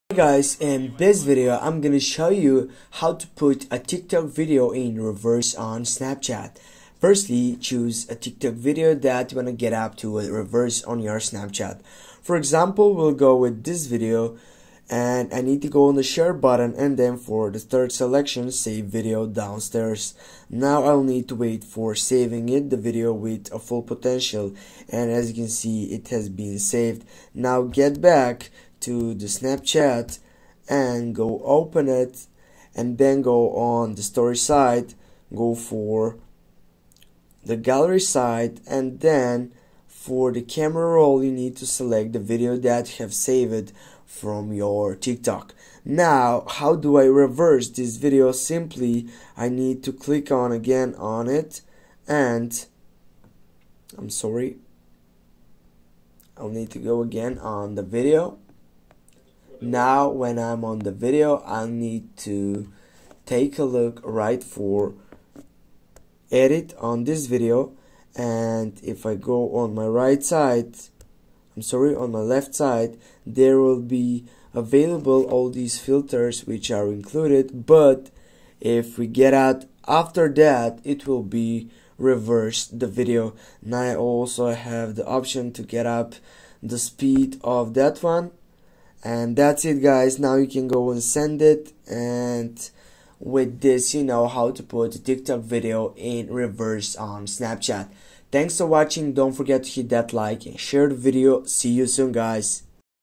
How to put a Tiktok Video in Reverse on Snapchat
This video will show you how to put a tiktok video in reverse on snapchat.